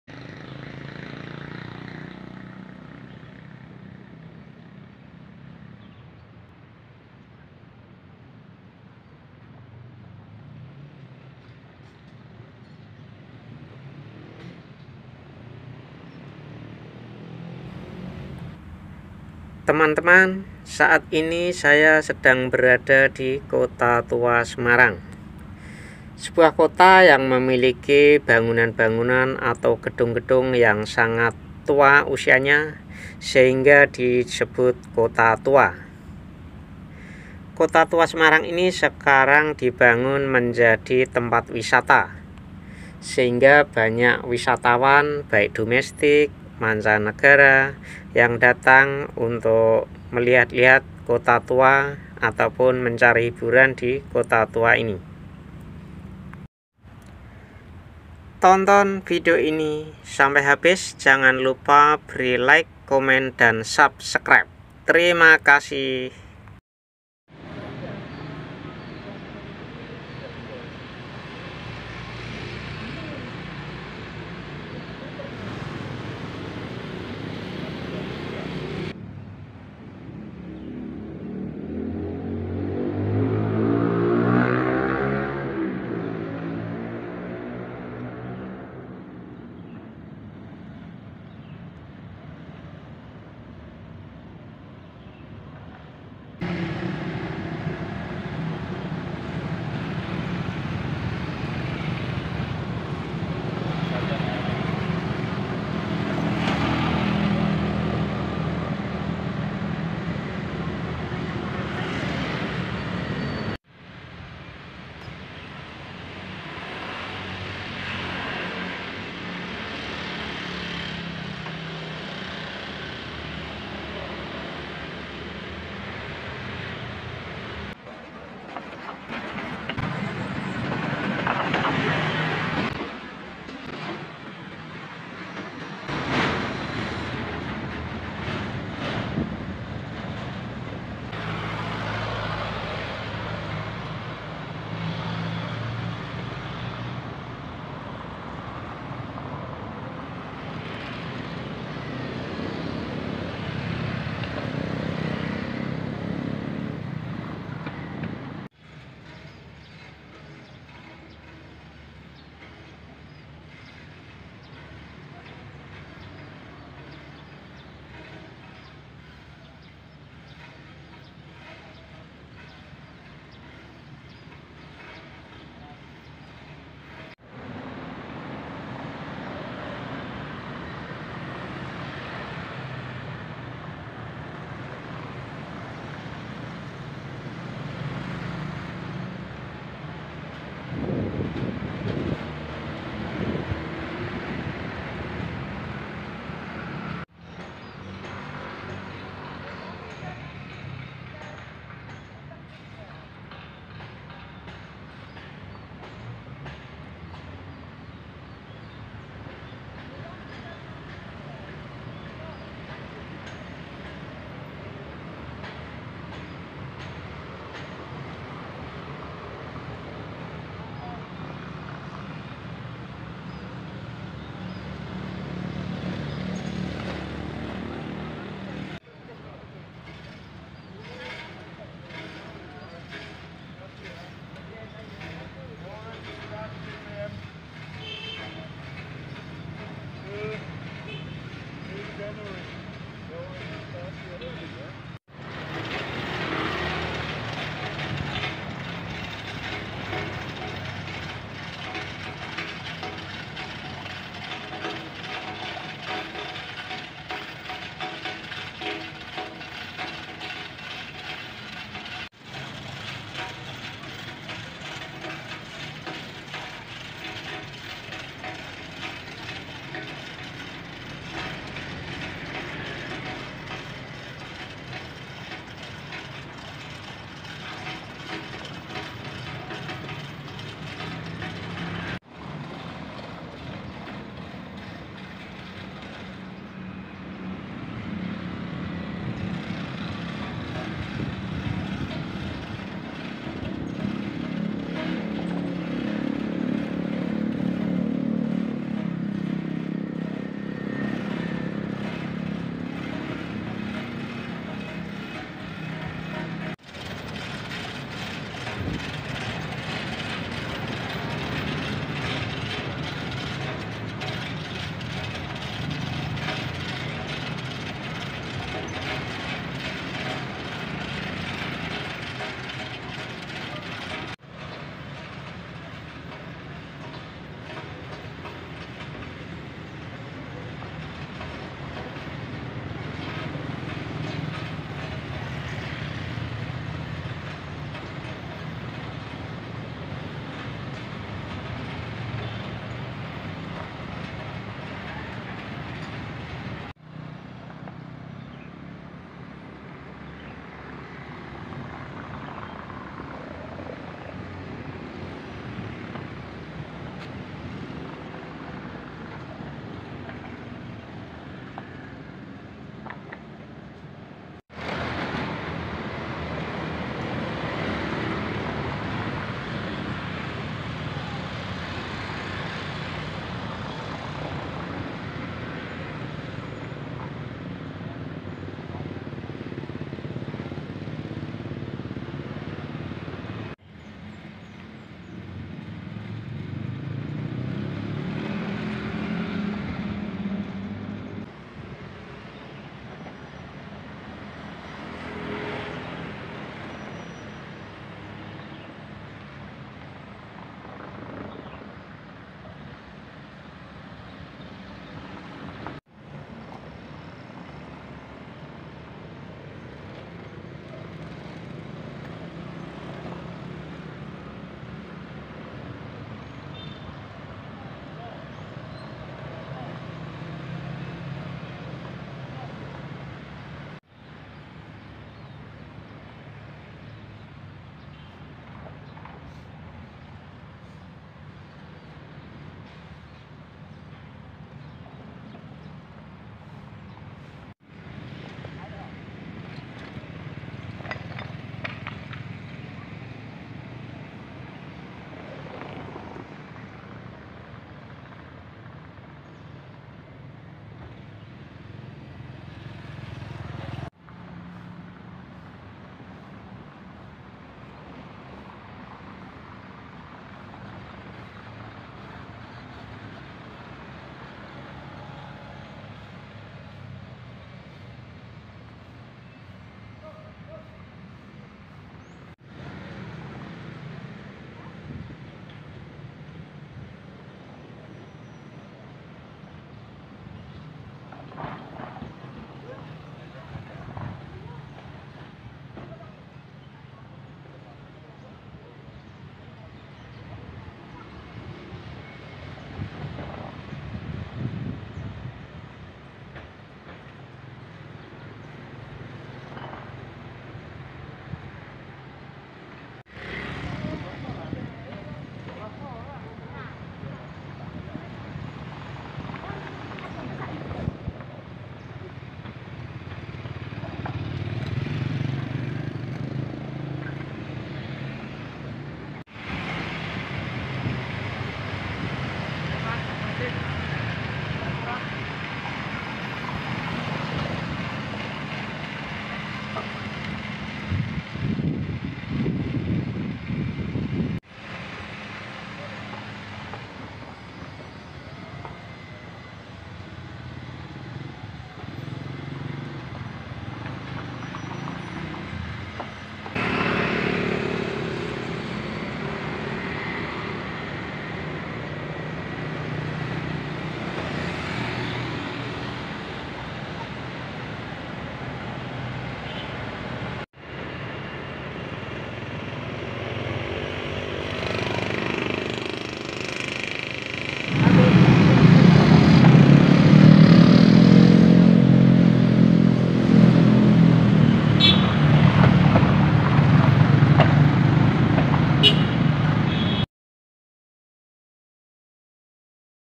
wisata Kota tua Semarang Jawa Tengah
Kota tua Semarang sebuah kota yang memiliki bangunan dan gedung2 yang sudah berusia sangat lama,, peninggalan zaman Belanda,,,sekarang dibangun menjadi tempat wisata yang sangat indah,,banyak wisatawan yang datang baik domestik maupun manca Negara,,,tonton video ini sampai habis jangan lupa like dan Subscribe,,,,♥️💙💖👍